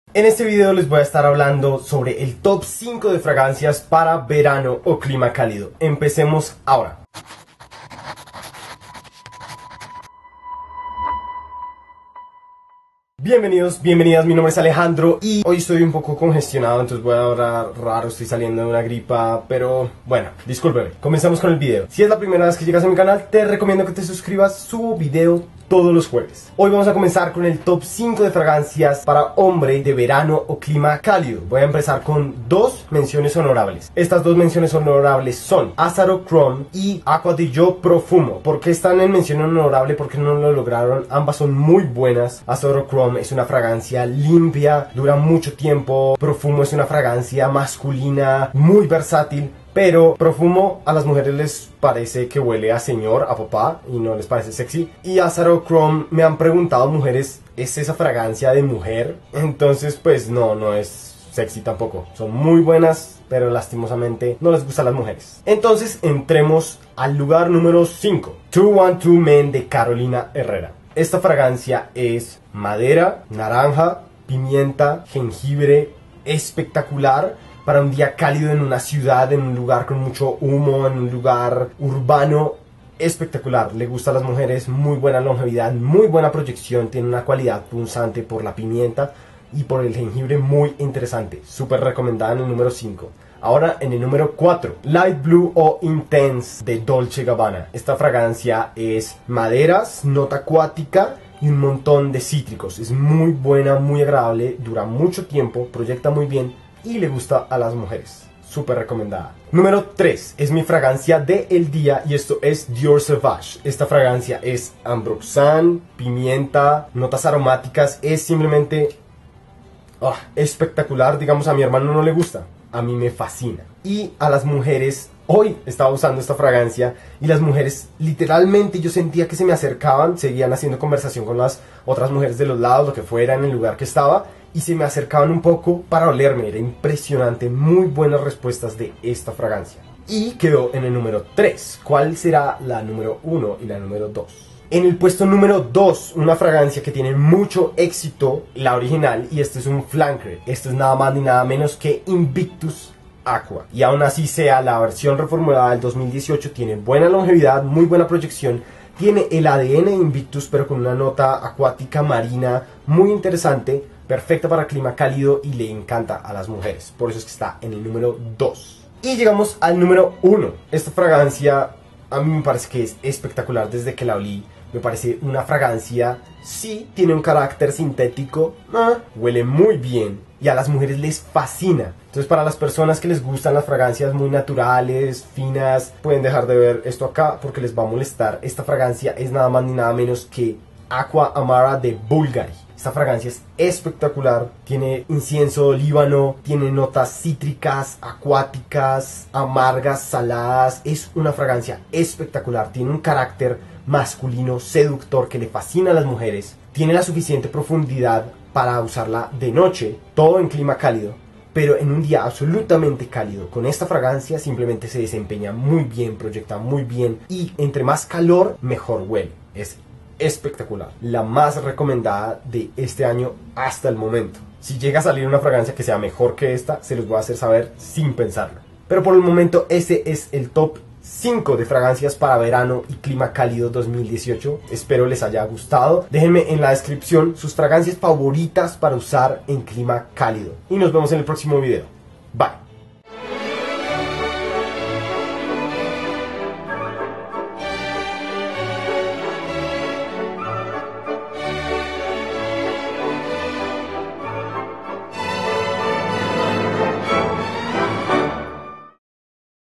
TOP 5 Fragancias Para Verano - 2018
Holaa! Hoy les traigo el TOP 5 Fragancias Para Clima Caliente 2018!!! (Hasta la Fecha)

¿TE GUSTA MI TRABAJO Y QUIERES APOYARME?

Puedes hacerme un aporte usando alguno de estos links!

Aqua Amara 100ml $61 USD

Invictus Aqua 100ml $63 USD

Dior Sauvage 100ml $102 USD

Light Blue eau Intense 100ml $71 USD

212 Men 100ml $54 USD

Acqua Di Gio Profumo 75ml $77 USD

Azzaro Chrome 50ml $ USD67

Mi Fragancia favorita hasta el momento